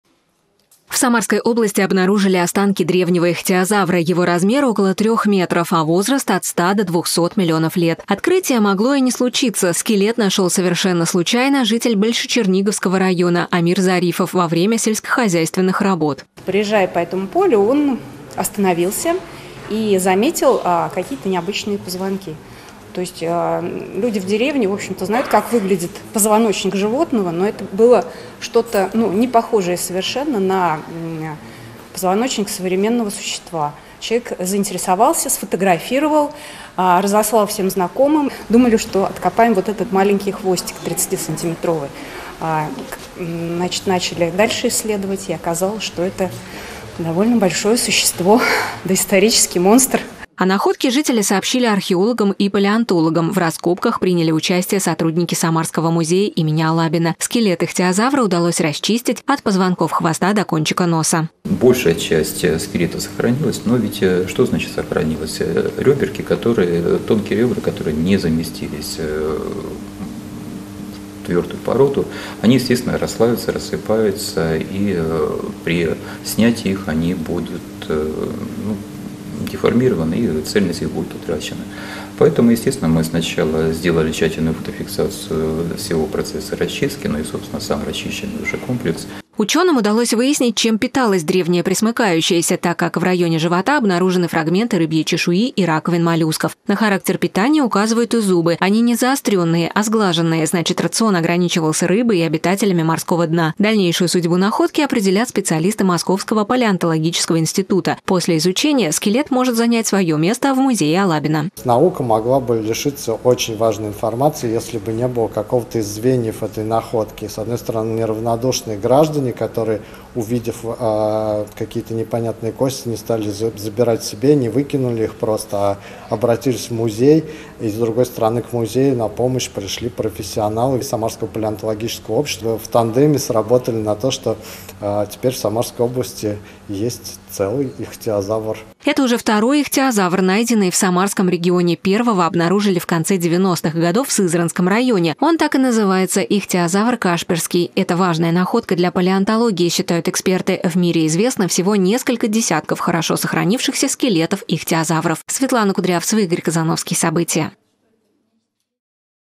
В Самарской области нашли останки ихтиозавра
В Самарском музее Алабина рассказали подробности находки скелета древнего Ихтиозавра, возраст которого - от 100 до 200 млн. лет. Как оказалось, механизатор одного из фермерских хозяйств Большечерниговского района Амир Зарифов во время вспашки поля обнаружил останки неизвестного существа. Он обратился к археологам, и они отправились вместе на раскопки. Оказалось, что находка действительно уникальна. Подробности расскажем далее.